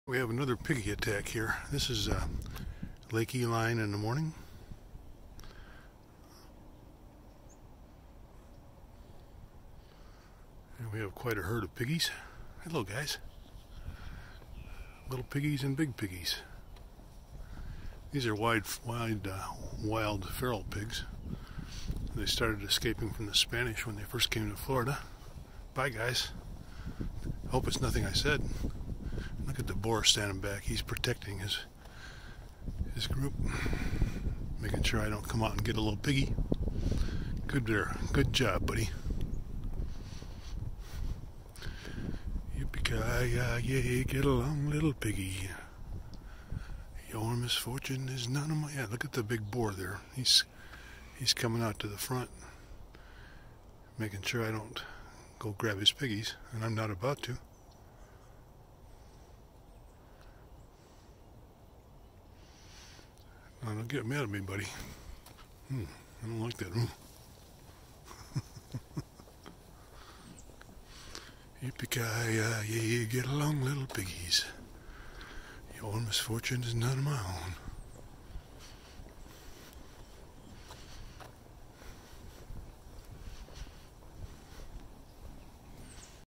Passel of Pigs Dec 27, 2020
Nice little Passel of Pigs Dec 27, 2020

Question: What does one call a group of hogs?

Answer: A group of pigs is called a drift or drove.
A group of young pigs is called a litter.
A group of hogs is called a passel or team.
A group of swine is called a sounder.
A group of boars is called a singular.